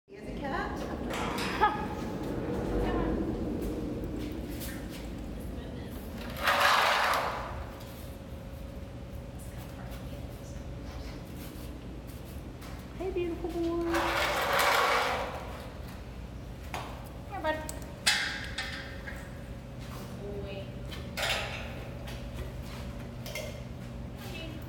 Sohan the Bengal Tiger Receives Treat from Megan Animal Care Specialist Busch Gardens
Sohan Receives Treat from Megan Animal Care Specialist Busch Gardens